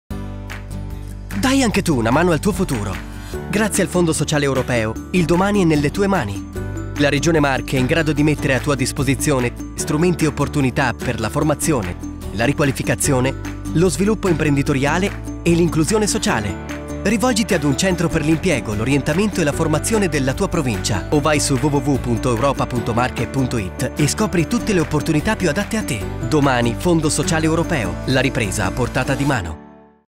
FSE, e il domani è nelle tue mani
Grazie al Fondo Sociale Europeo la Regione Marche mette a tua disposizione strumenti e opportunità per formazione, riqualificazione, sviluppo imprenditoriale e inclusione sociale.